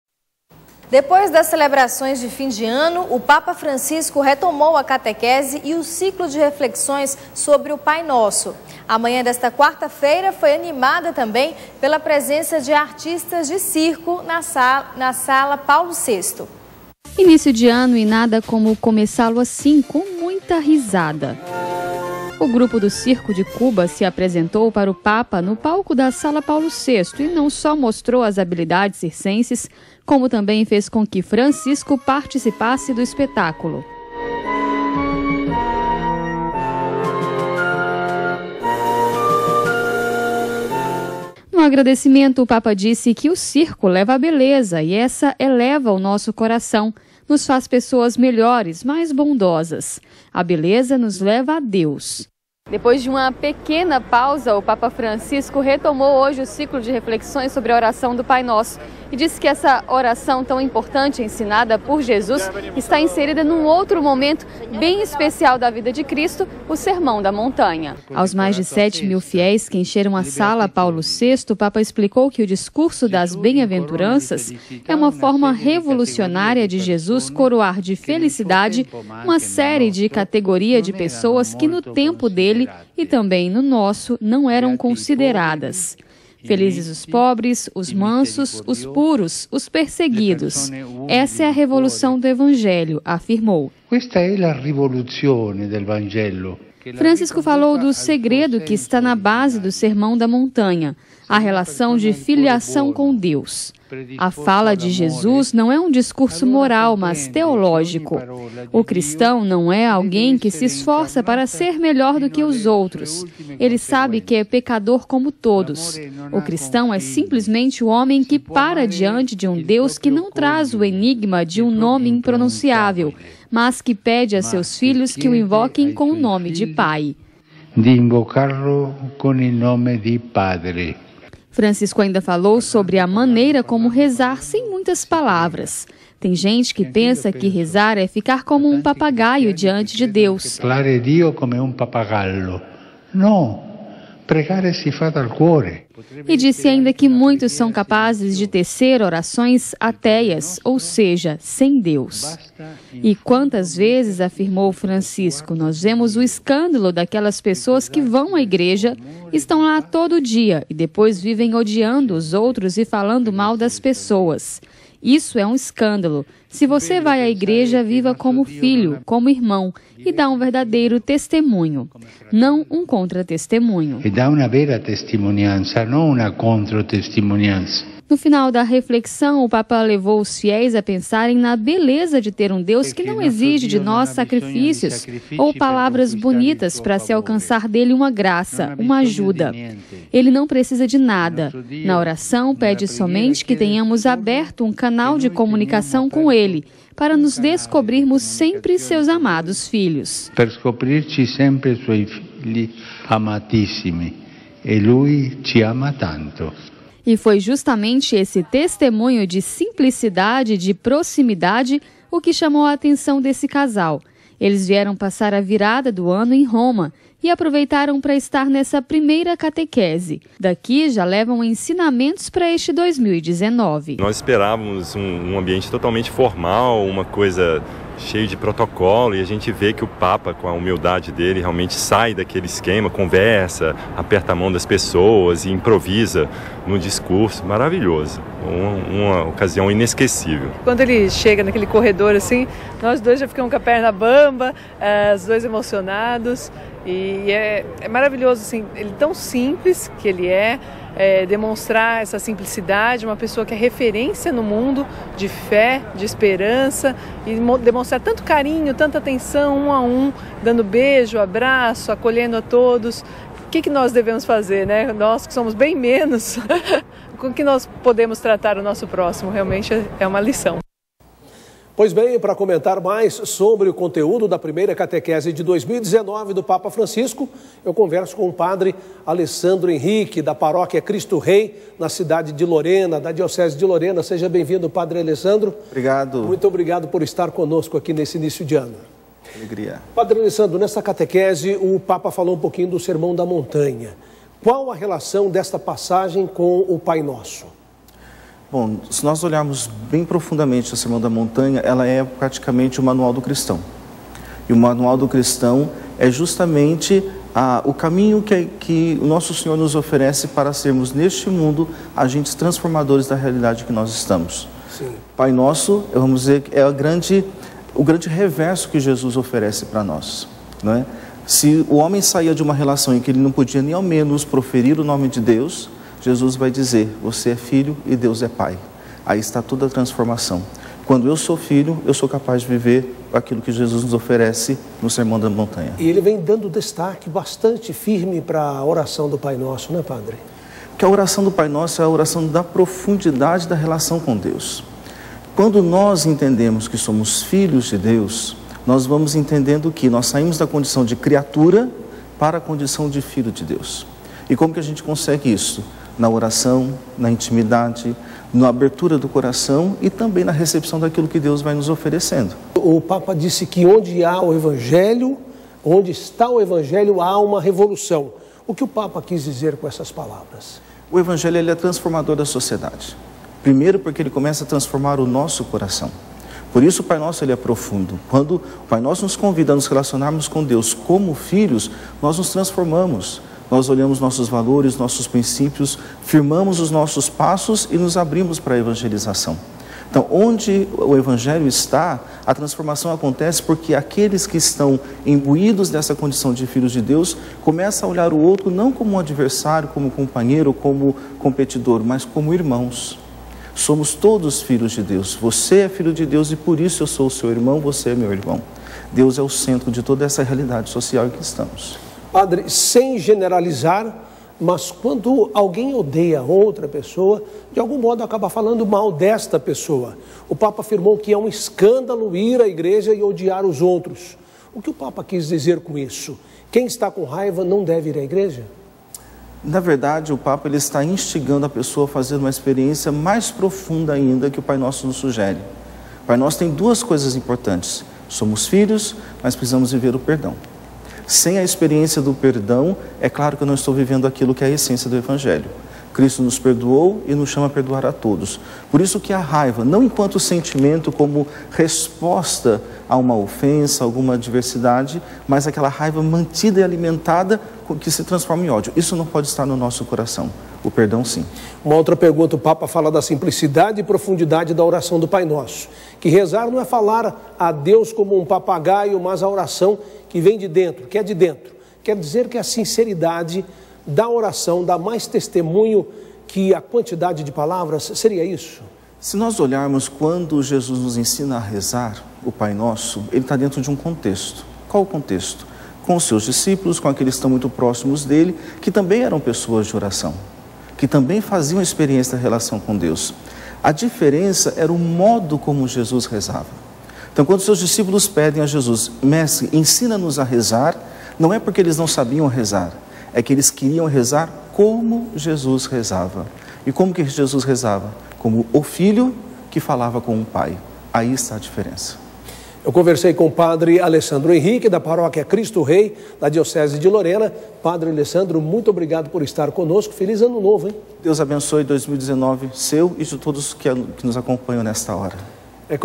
Papa Francisco celebra a primeira Catequese de 2019
Depois das celebrações de fim de ano, o Papa retomou a Catequese e o ciclo de reflexões sobre o Pai Nosso. Na manhã desta quarta-feira, 3, contou também com a presença de artistas de circo na Sala Paulo VI.

Reportagem de Lízia Costa e Joseano Marques